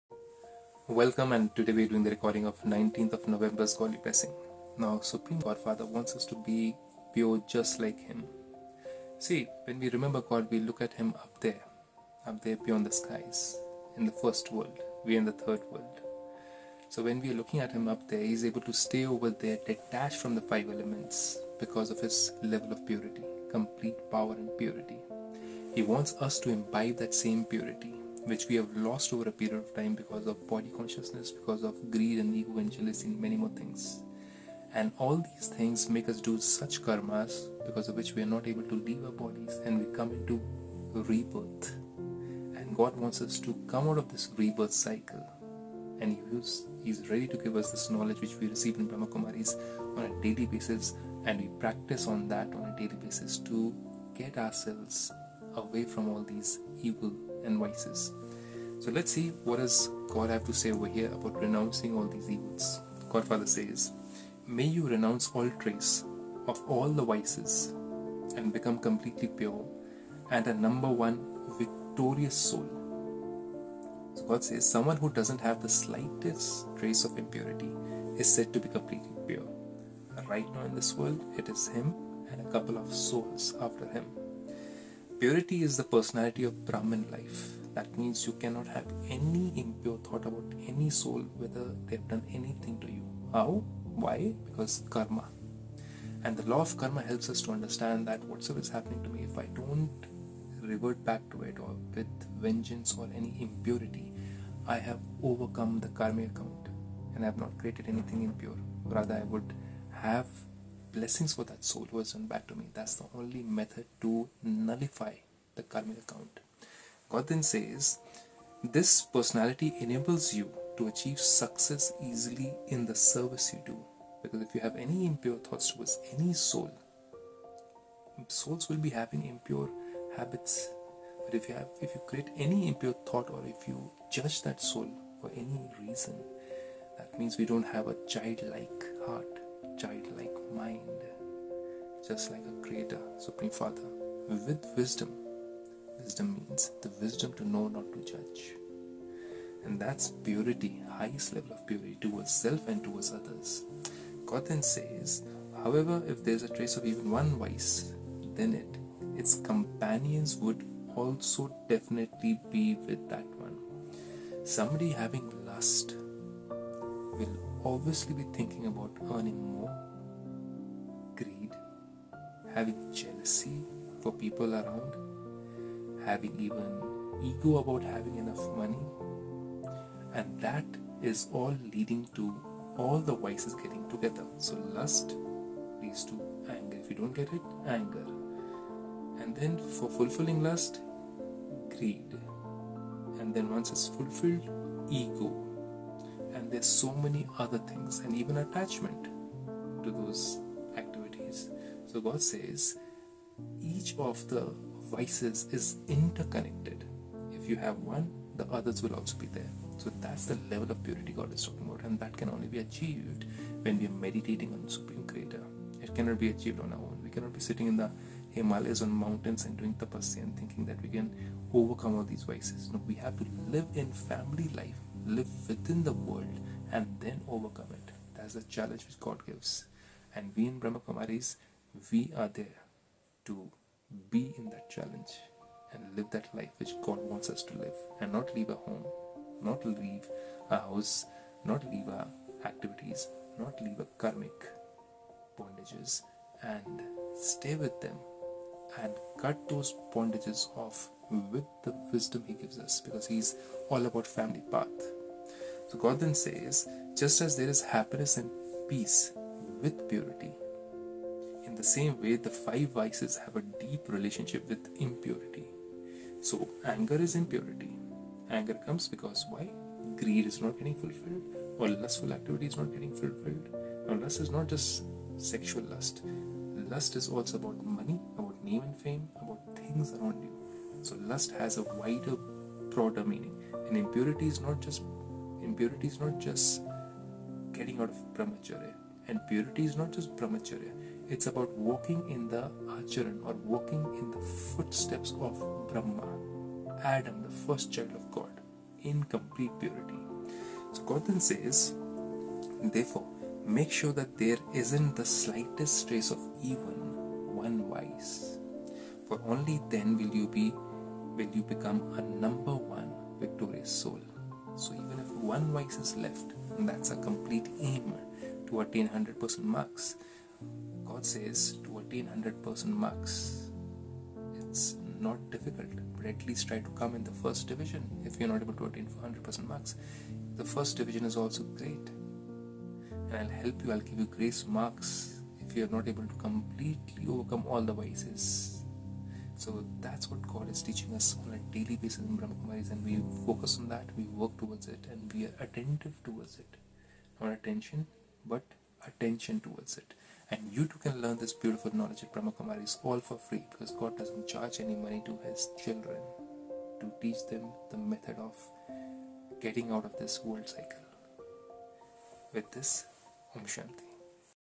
Renounce all trace of all the vices and become completely pure and a number one victorious soul.
Day 292 May you renounce all trace of all the vices and become completely pure and a number one victorious soul.

Om Shanti .